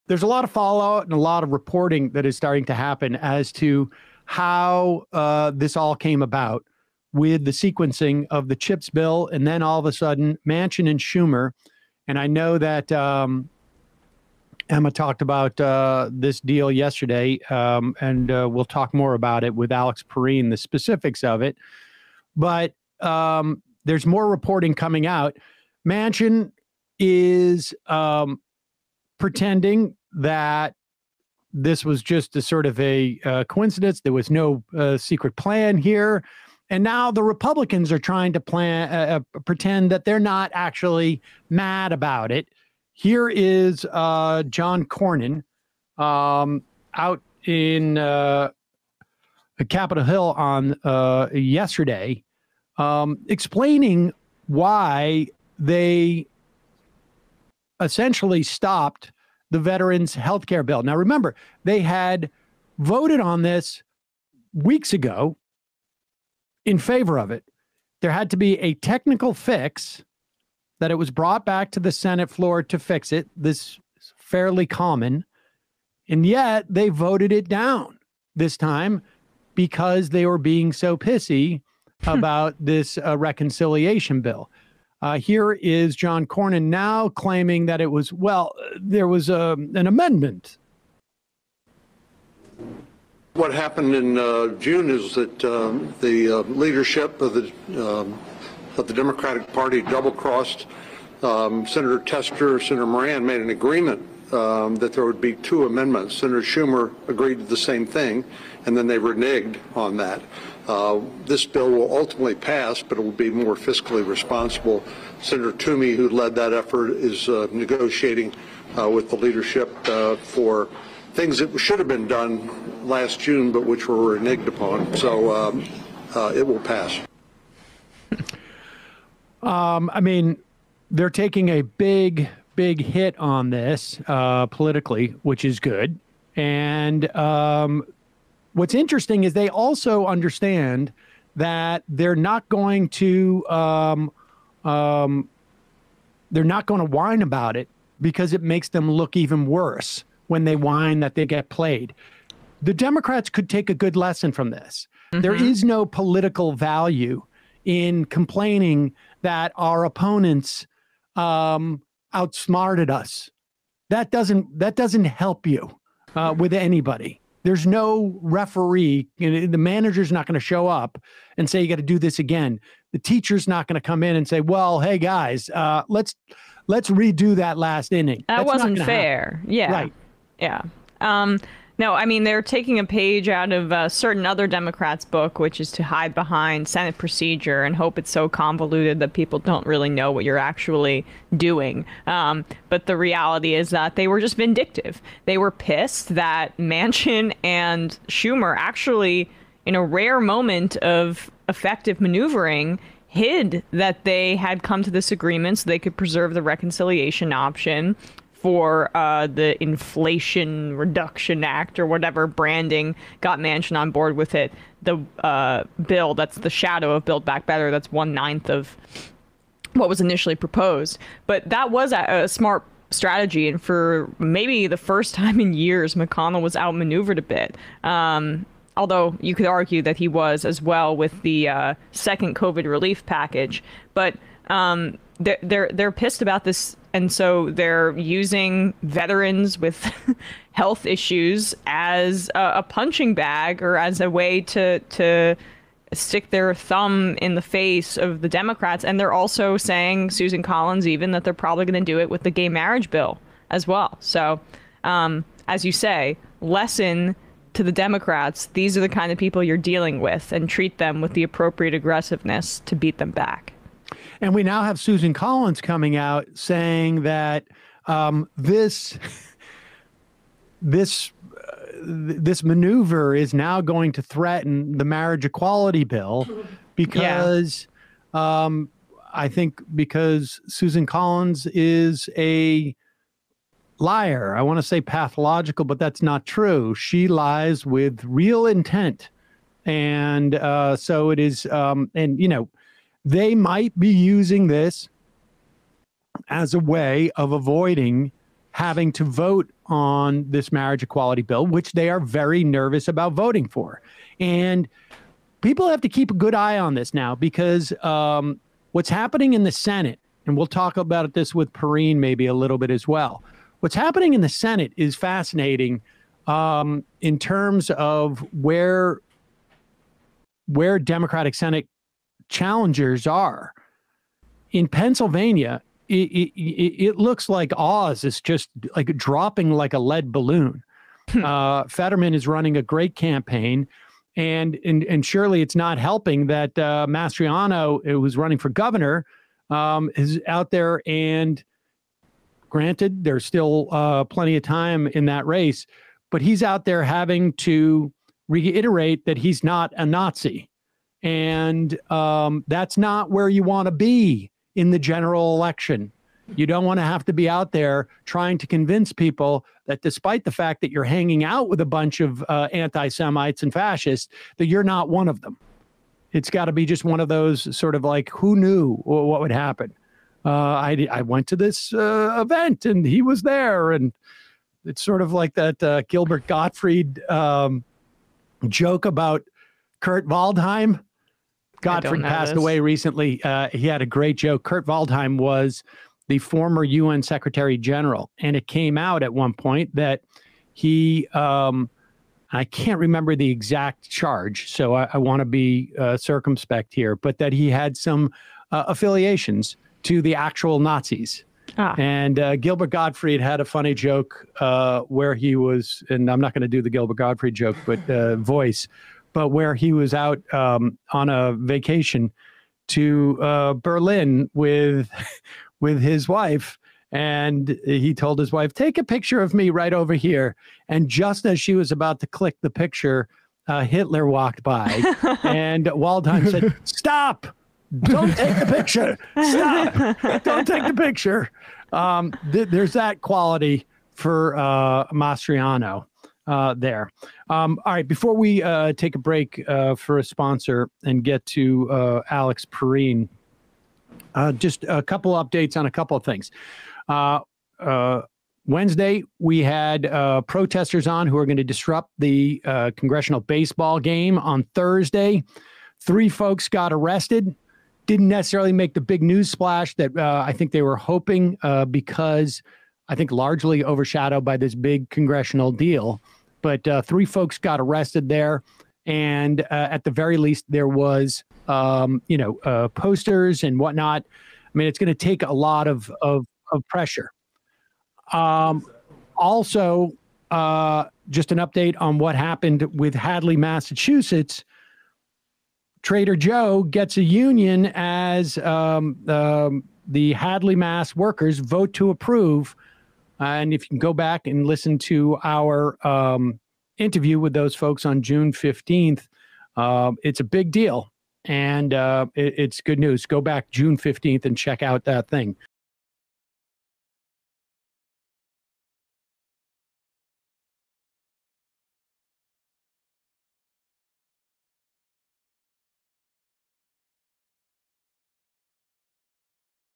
GOP Mask Is Off After Holding Healthcare For Military Veterans Hostage
Sam and Emma dive into legislative updates from the Senate, from the Manchin-Schumer Inflation Reduction Act deal and its backlash, to the GOP putting a stop to healthcare for veteran victims of agent orange, with the latter largely coming from the GOP’s desire to punish democrats for the former (with Same-Sex Marriage reform also a potential right-wing punching bag).